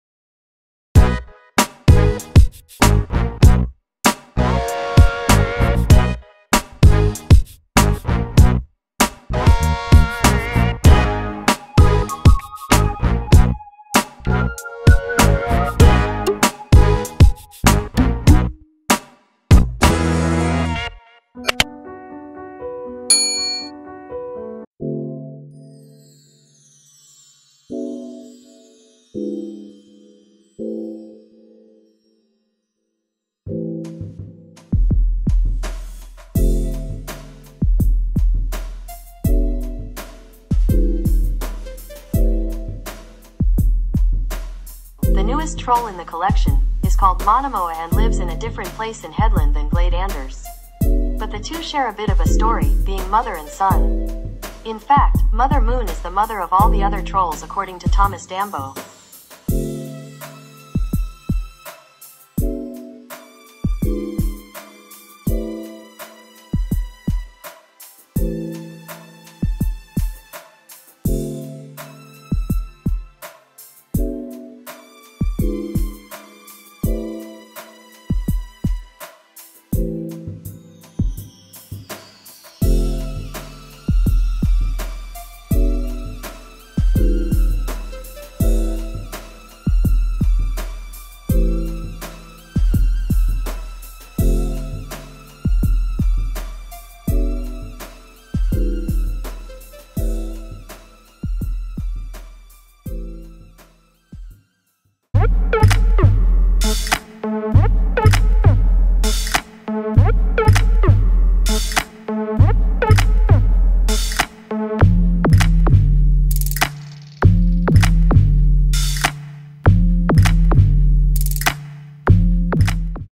Månemor troll #100
The jubilee troll – the newest troll in the collection – is called Monemowa and lives in a different place in Hedeland than Glade Anders. But the two share a bit of a story, being mother and son. In fact, Mother Moon is the mother of all the other trolls according to Thomas Dambo.

🔴  I HAVE MORE INTERESTING VIDEOS PLEASE CHECK IT OUT.